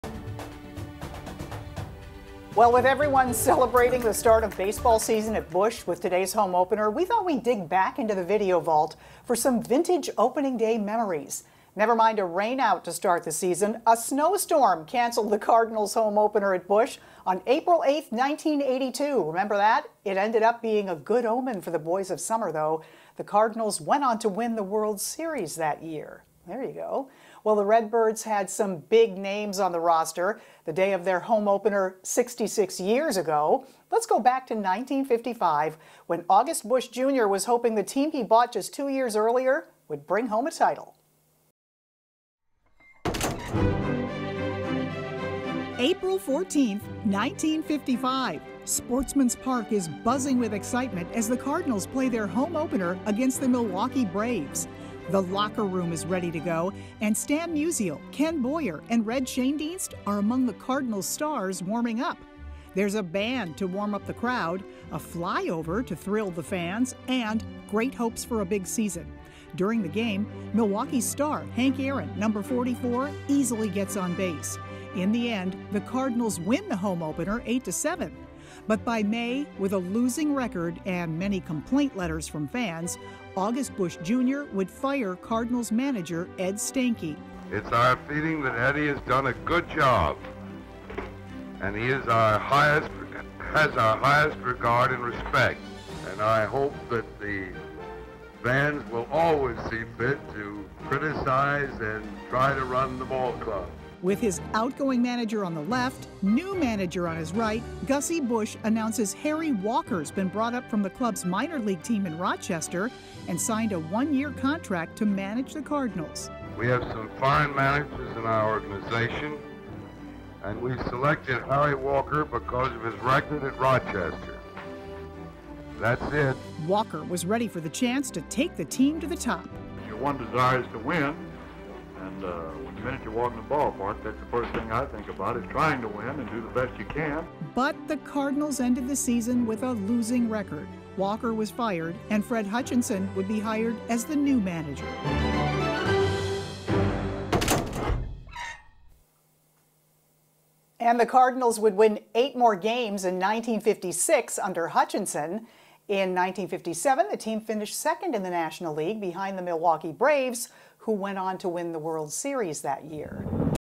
Vingate KSDK | With Musial, Schoendienst on roster, hopes were high for 1955 Cardinals
ST. LOUIS - In the spring of 1955, August Busch Jr. was hoping the team he bought two years earlier would bring home a title.

The home opener was on April 14 that year. Sportsman's Park was buzzing with excitement as the Cardinals played the Milwaukee Braves.

The locker room was ready to go, and Stan Musial, Ken Boyer and Red Schoendienst were among the Cardinals' stars warming up.

A band was on hand to warm up the crowd, and a fly-over thrilled the fans, who had great hopes for a big season.

During the game, Milwaukee's star, No. 44 Hank Aaron, easily got on base.